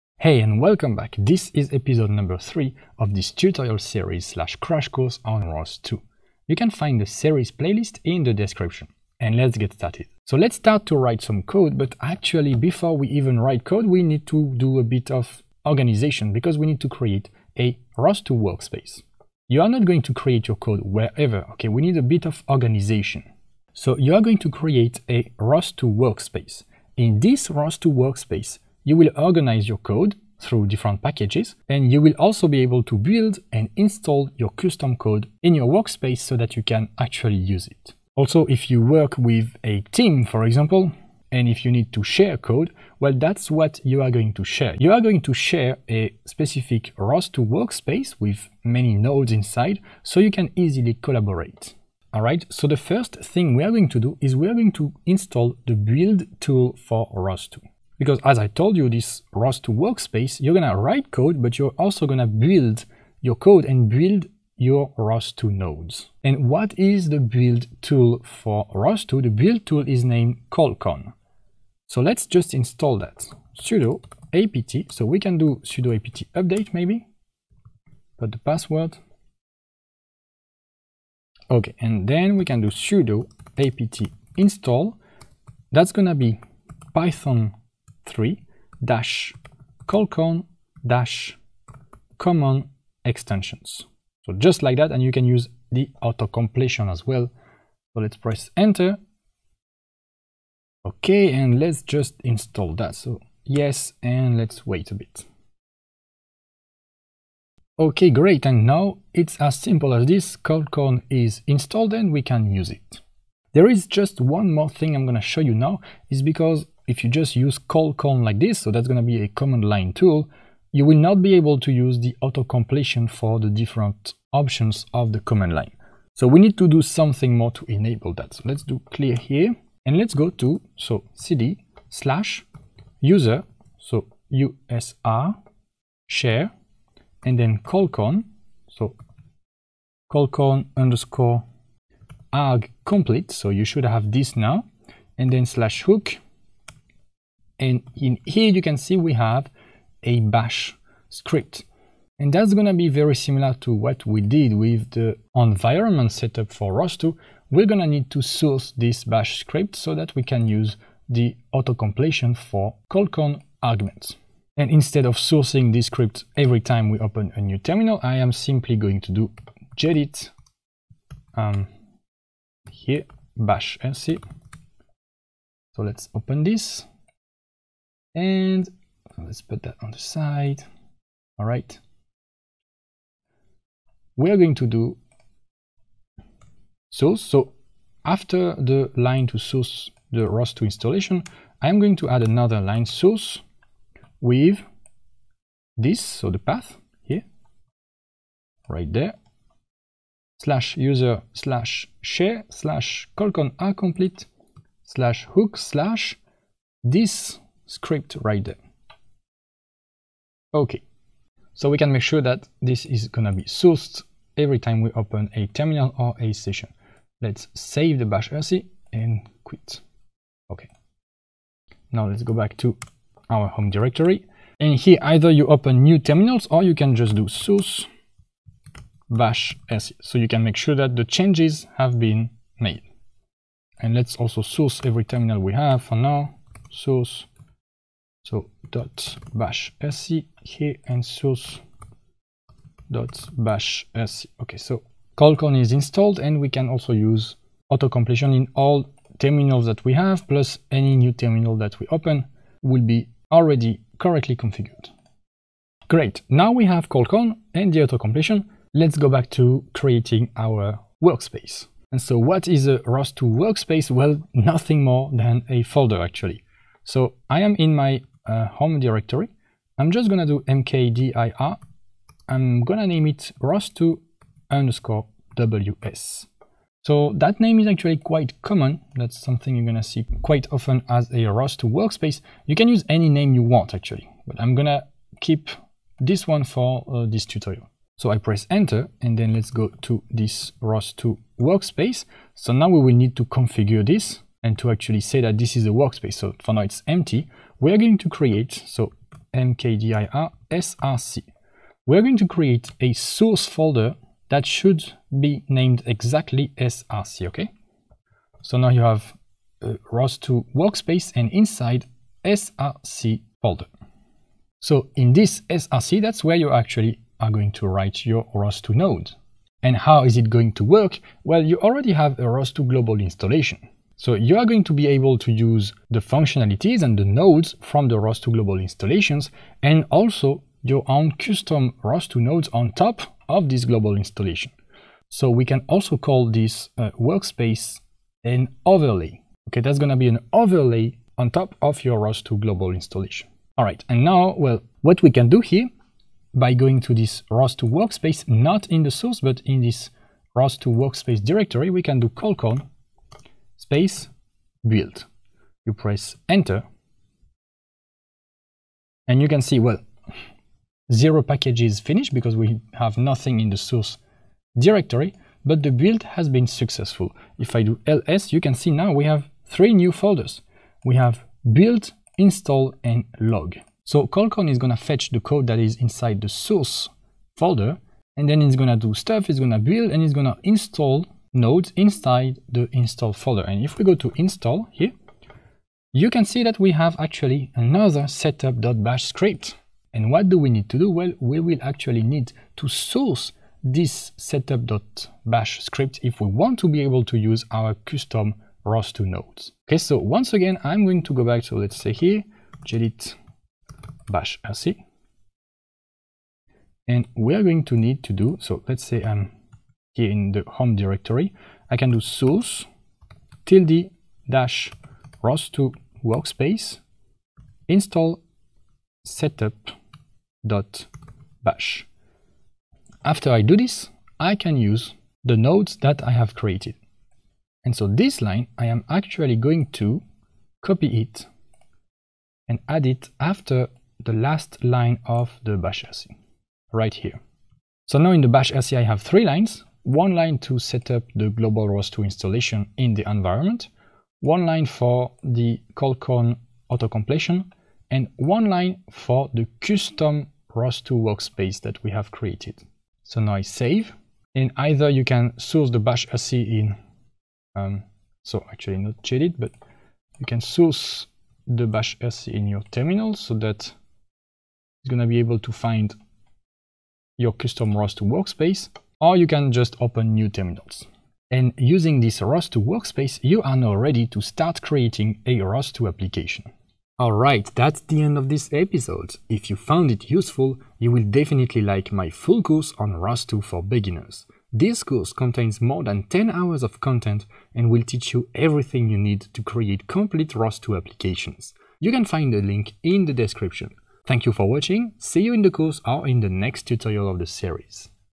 Create and Set Up a ROS2 Workspace - ROS2 Tutorial 3
In this ROS2 tutorial you will create and set up a ROS2 workspace, which will allow you to write custom ROS2 code.

⏱️ Chapters
(0:00) What’s a ROS2 workspace?
(0:52) Install colcon
(1:53) autocompletion
(4:15) Create a ROS2 workspace
(5:44) Build and source the workspace